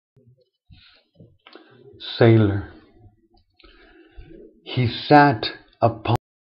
SAILOR
SAILOR written by Langston Hughes, read by homosexual poet Hektor Munoz, filmed by Munoz on Sunday, October 3rd, 2010 in Denver, Colorado.